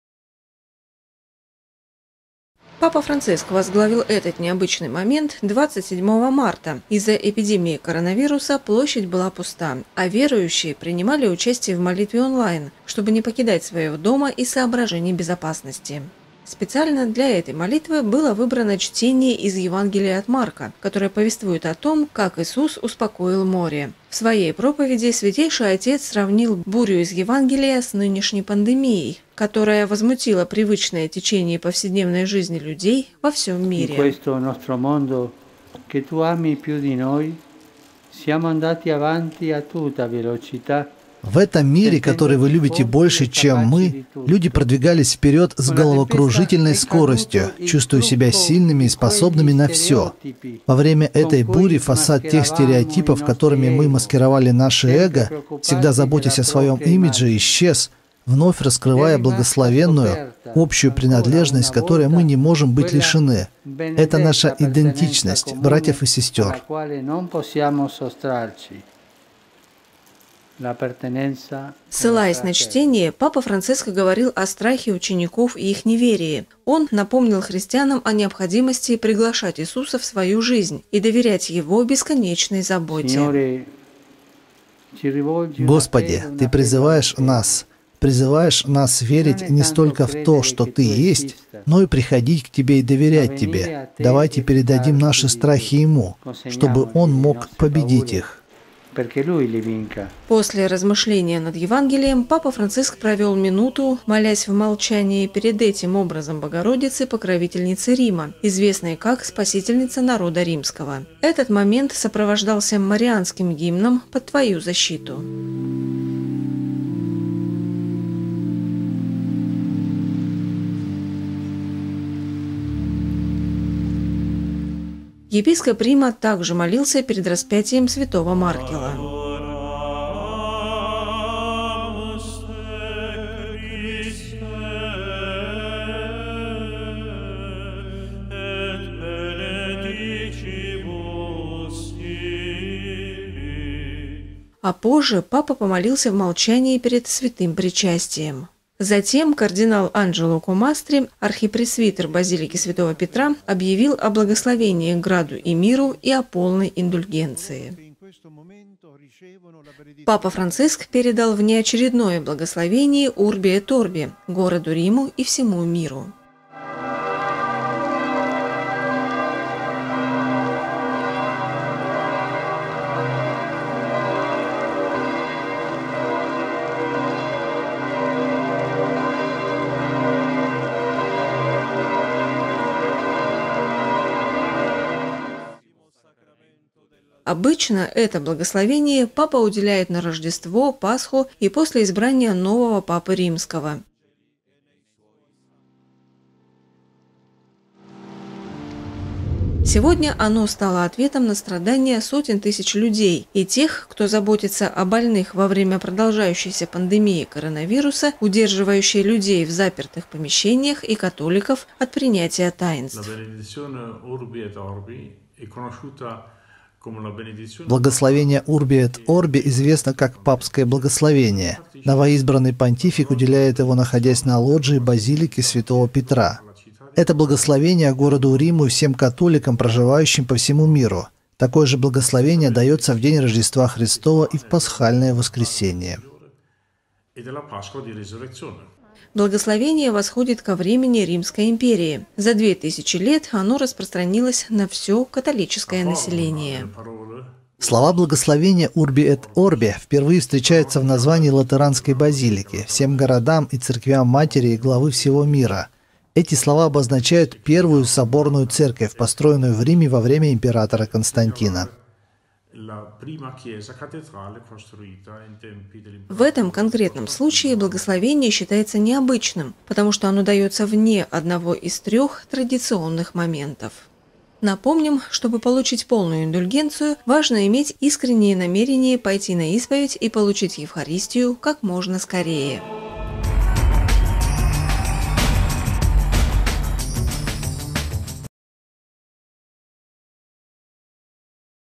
Папа Франциск уделил внеочередное благословение Urbi et оrbi
27 марта Папа Франциск уделил внеочередное благословение Urbi et оrbi. Из-за эпидемии коронавируса площадь была пуста, а верующие принимали участие в молитве онлайн, чтобы не покидать своего дома из соображений безопасности.

Специально для этой молитвы было выбрано чтение из Евангелия от Марка, которое повествует о том, как Иисус успокоил море. В своей проповеди Святейший Отец сравнил бурю из Евангелия с нынешней пандемией, которая возмутила привычное течение повседневной жизни людей во всем мире.